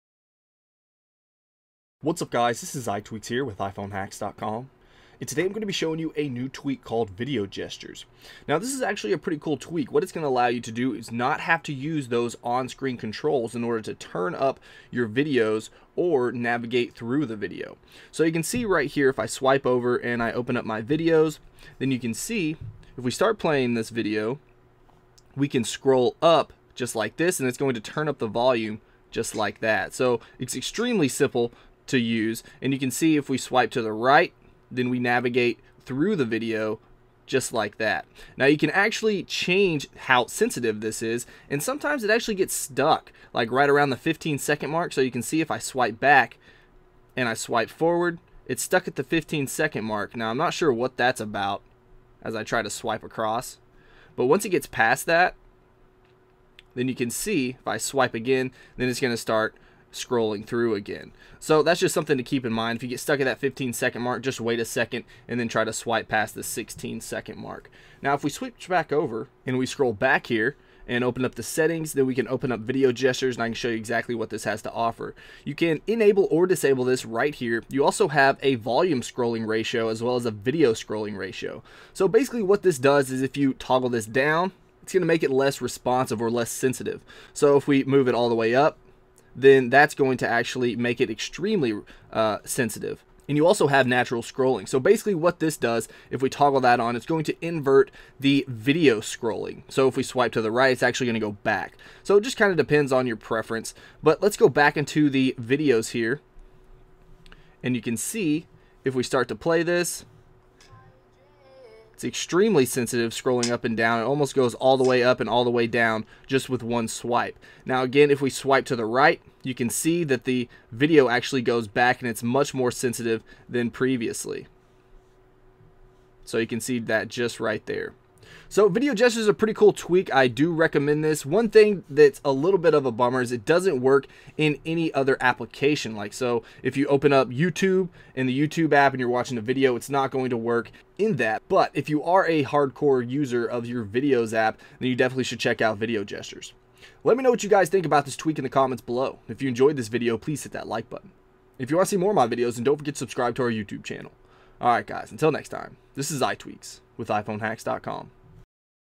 VideoGestures allows you to control videos with simple gestures - iPhone Hacks
Here's a nice tweak that allows you to control videos with simple gestures. You also have a few settings to configure in the settings panel.